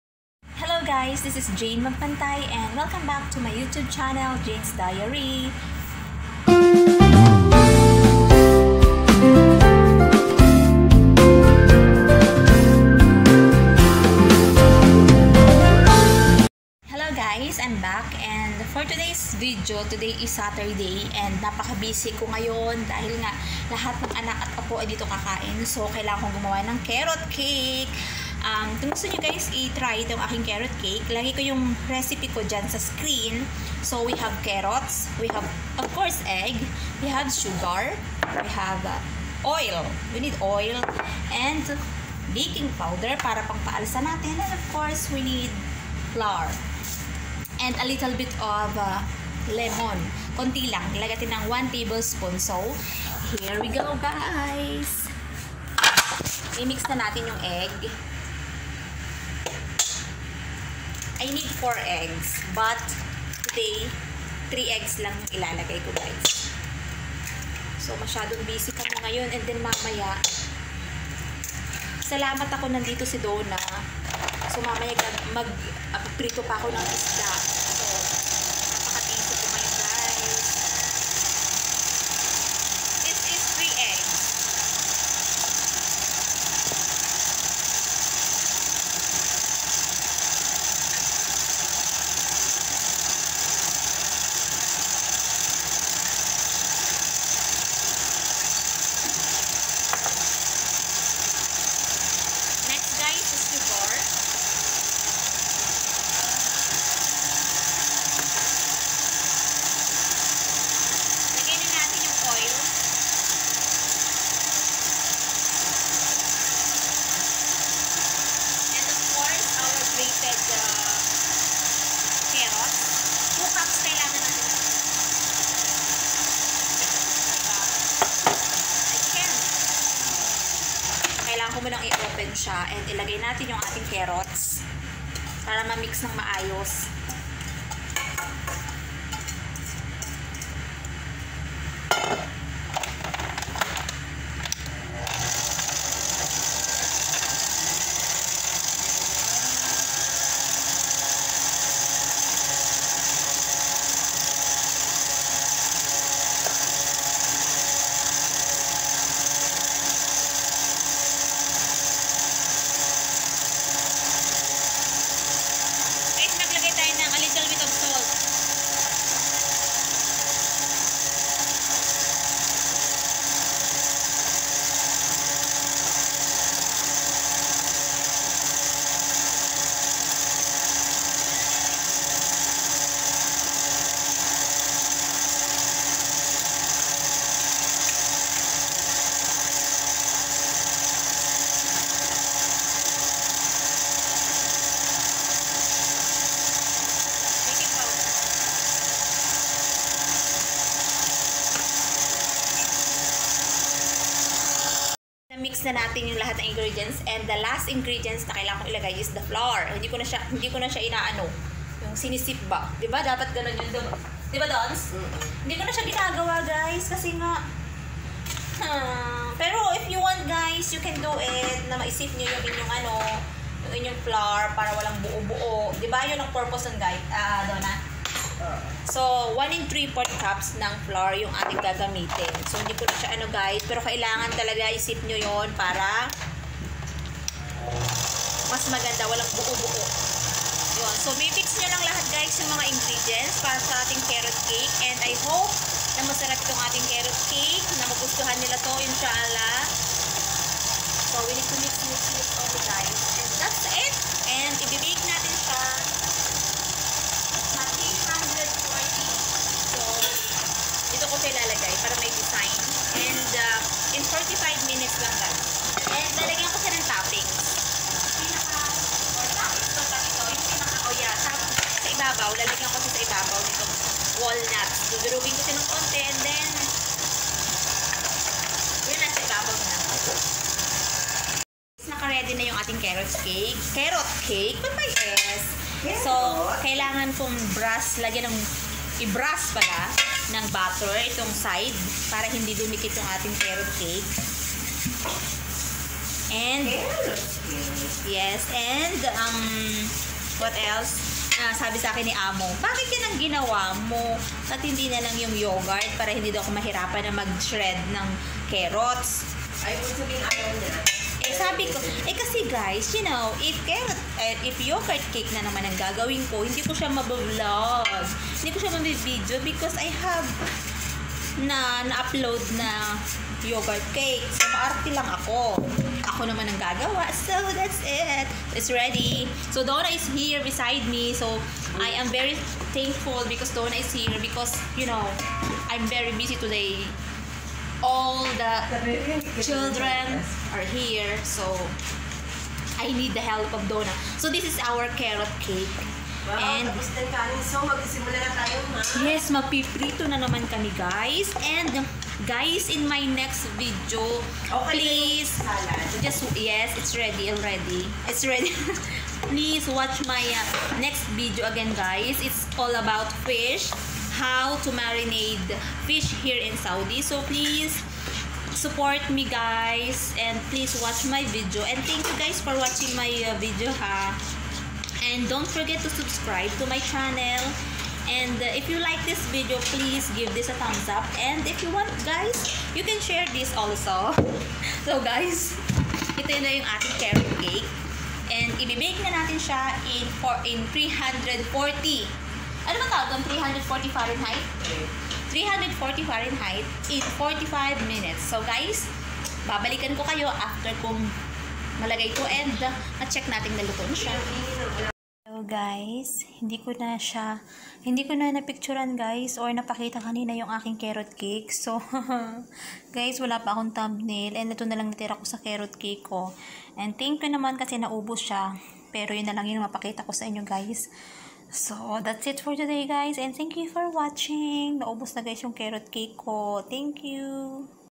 CARROT CAKE/jane magpantay
Hello guys!!!
Gagawa po ako ngaun ng carrot cake,kase marami bisita ngaun ang amo ko.